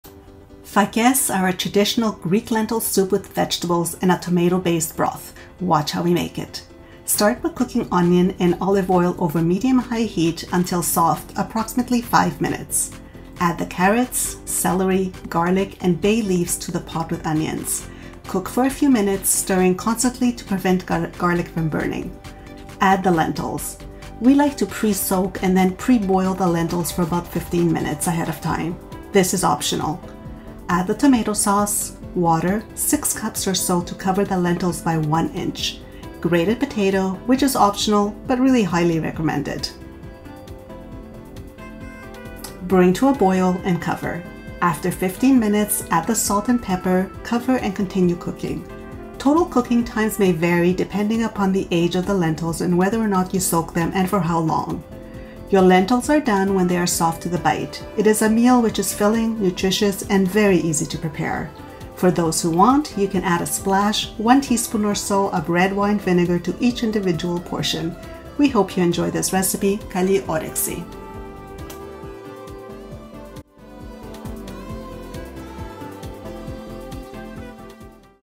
Fakes, a Greek lentil soup recipe
A staple in our homes. See how easy it is to make a delicious pot of Greek lentil soup, or fakes. It’s no surprise that this wholesome vegan soup has been made almost every Friday by our dad. We look forward to it still!